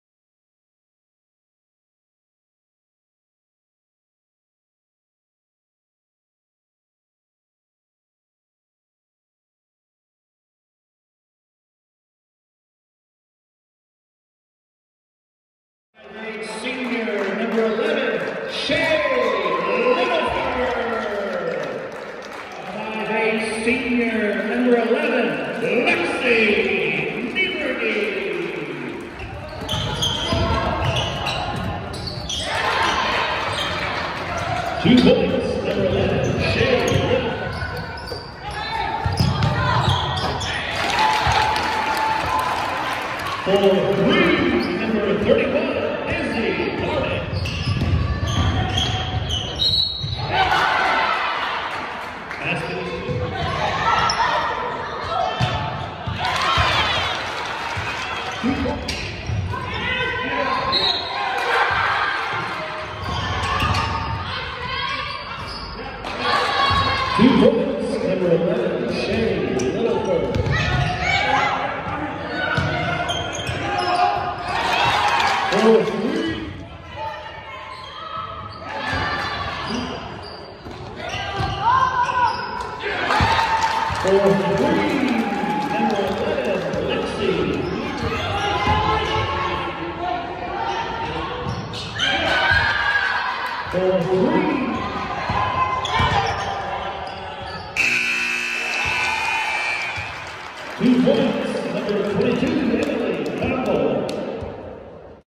Highlight Reel- Charleston Lady Trojans @ Lady Shoes (1st QTR Highlights) Charleston wins 67-56!
Charleston @ Teutopolis Lady Shoes 🤘